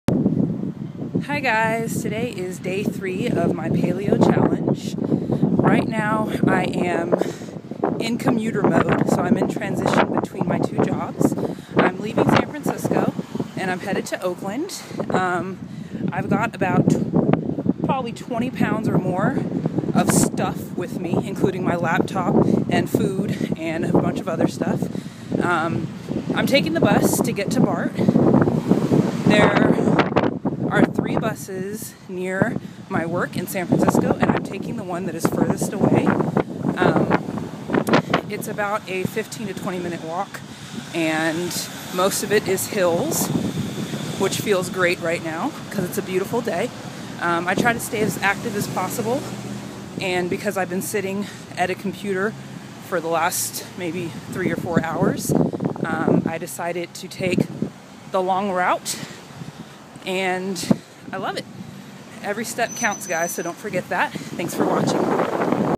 Paleo Challenge Day 3
See one example of how I take every opportunity I can to get my body moving!